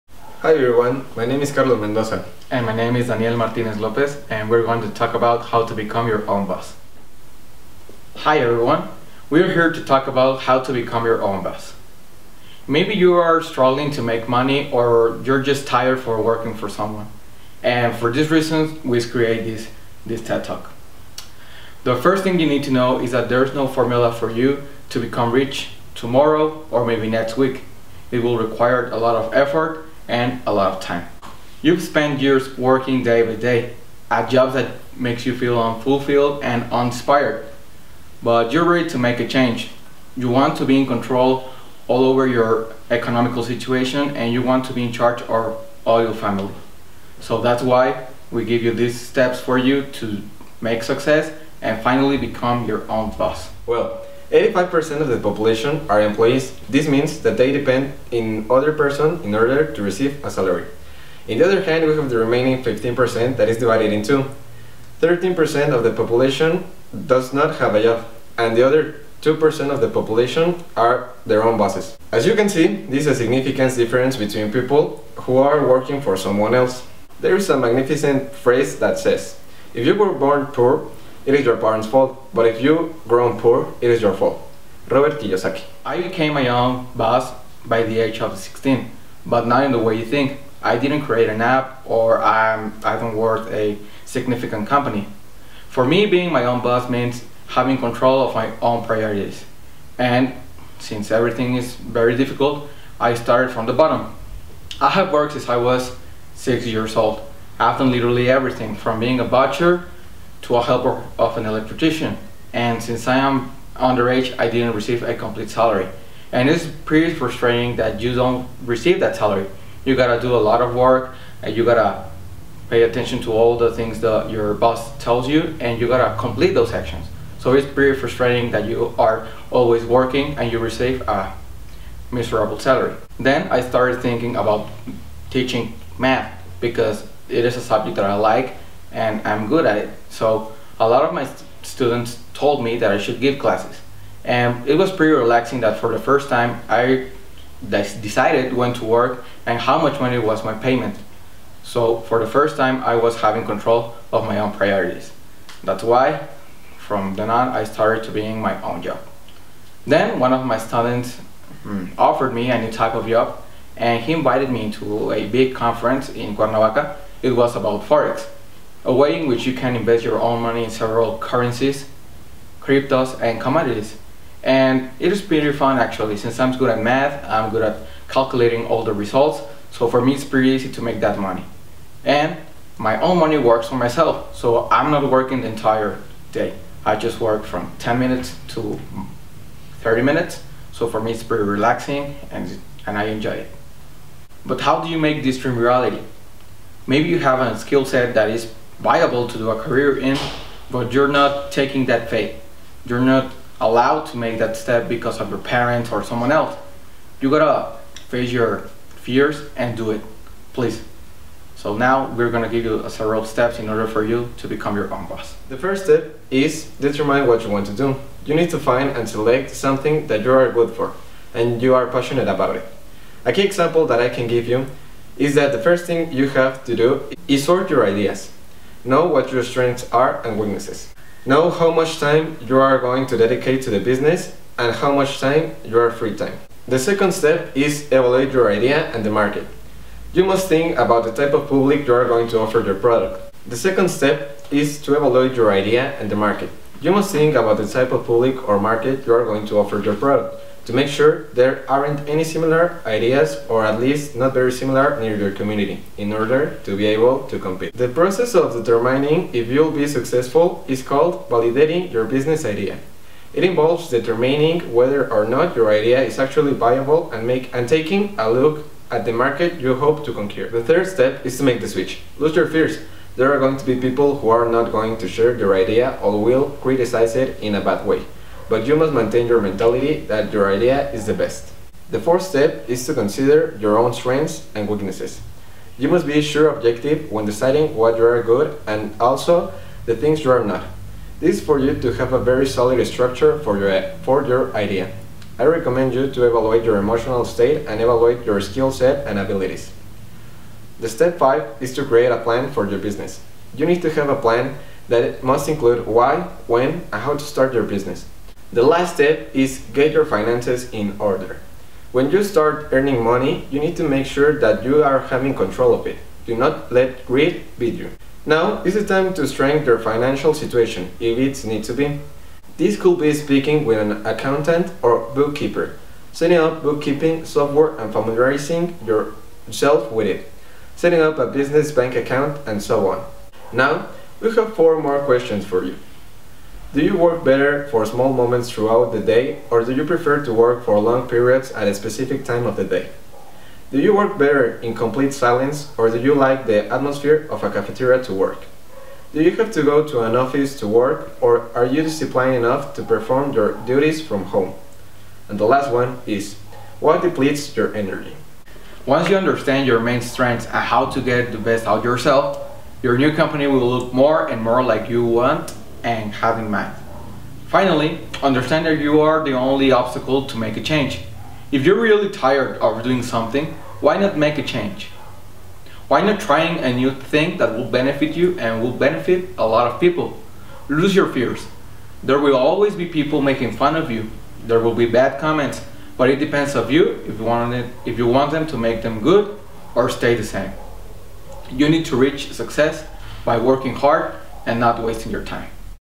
How to become your own Boss
Students
Daniel Martínez
Carlos Emiliano Mendoza Álvarez

In this ted talk you will find some steps that will guarantee you to be able to organize your times and work for yourself.